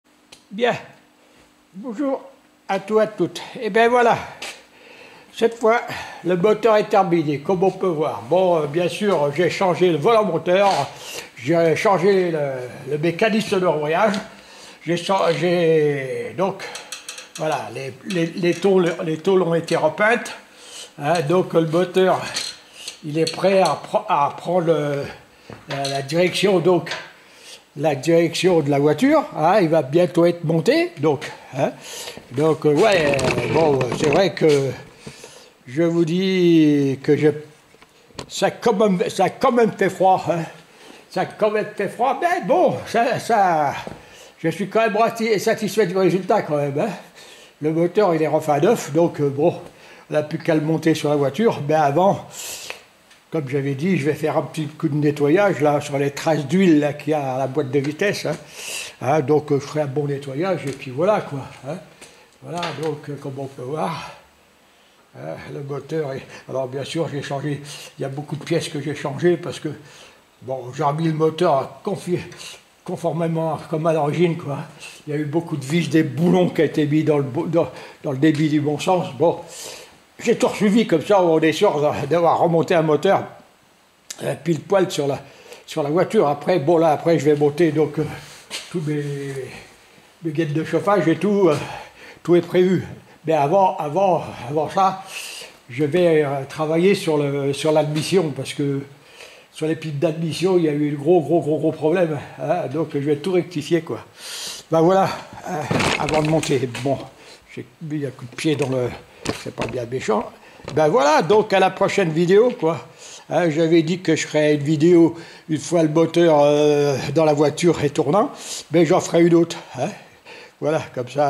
La Panhard 24 Bt de 1967 à notre ami Eric N° 1- La présentation du moteur une fois réparé
J'ai deux petites minutes pour présenter le moteur M8s de la Panhard 24bt  habillé et prêt à être remonté sur la voiture.
Deux minutes avant de commencer son implantation dans le compartiment, l'accoupler à la boite à vitesses.
Fortement détériorés par la rouille, le volant moteur et le mécanisme d'embrayage ont été remplacés, ainsi la boulonnerie et visseries non d'origines.
à ce stade du remontage, une bonne dizaines d'heures est nécessaire pour mener à bien la fin de cette mauvaise mésaventure ...